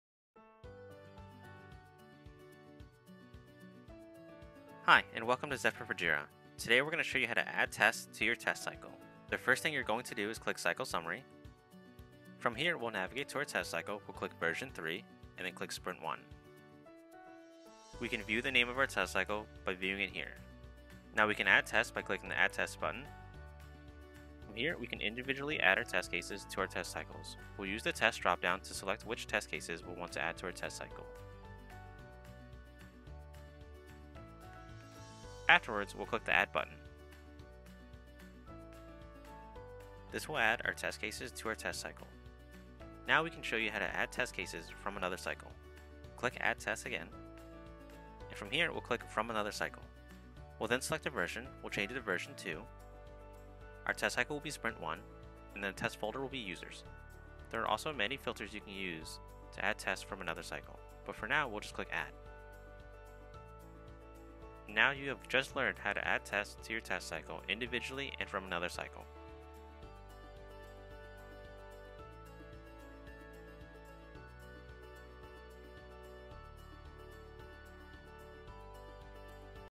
Adding Tests to Test Cycle in Zephyr for Jira Server & Data Center
When individual tests are added to test cycles, the tests can be easily viewed and assigned.